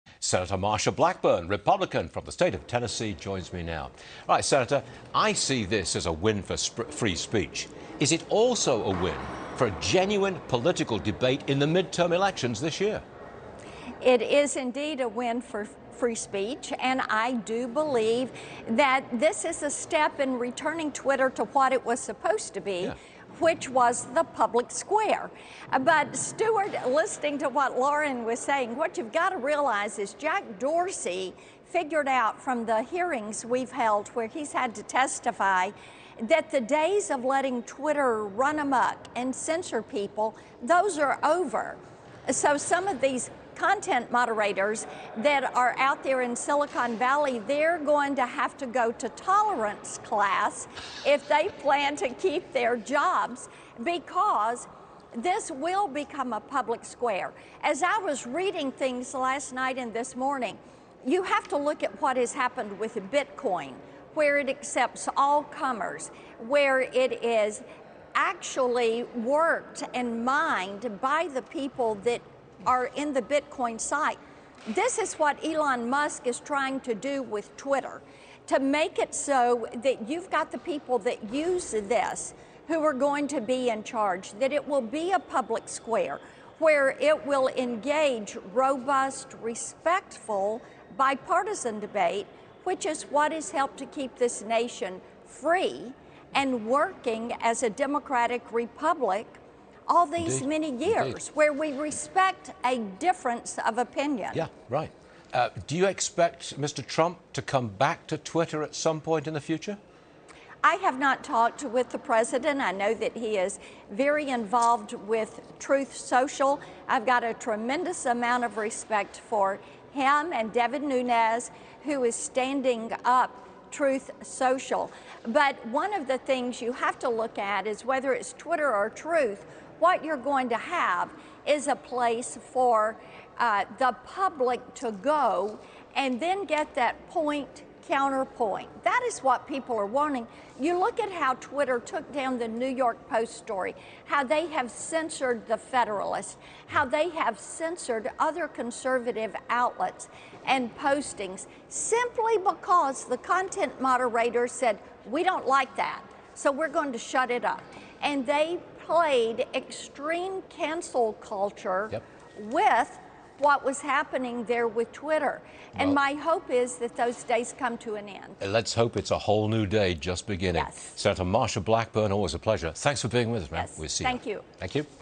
Sen. Blackburn: These days are coming to an end
Sen. Marsha Blackburn, R-Tenn., says Elon Musk's purchase is a step to returning Twitter to its original purpose.